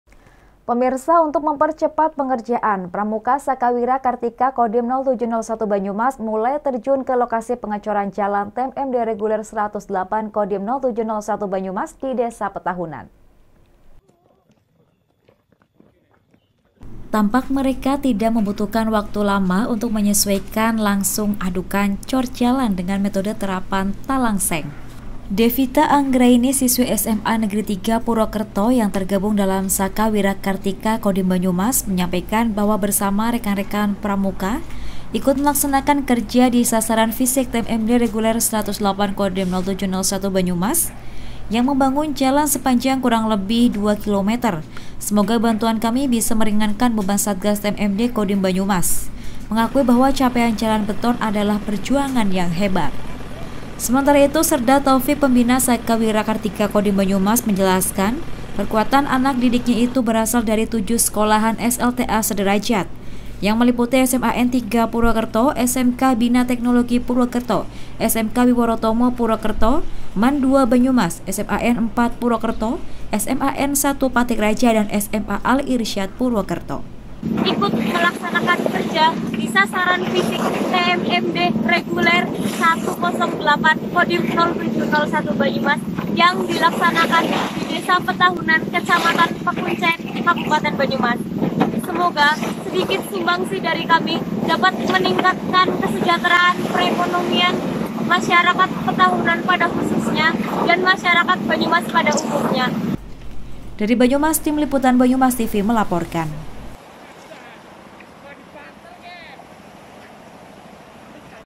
PRAMUKA MULAI DI TERJUNKAN KE LOKASI TMMD REGULER 108 KODIM 0701 BANYUMSA
UNTUK MEMPERCEPAT PENGERJAAN PRAMUKA SAKA WIRA KARTIKA KODIM 0701 BANYUMAS MULAI DITERJUNKAN KE PENGECORAN JALAN TMMD REGULER 108 KODIM 0701 BANYUMAS DI DESA PETAHUNAN KECAMATAN PEKUNCE KABUPATEN BANYUMAS.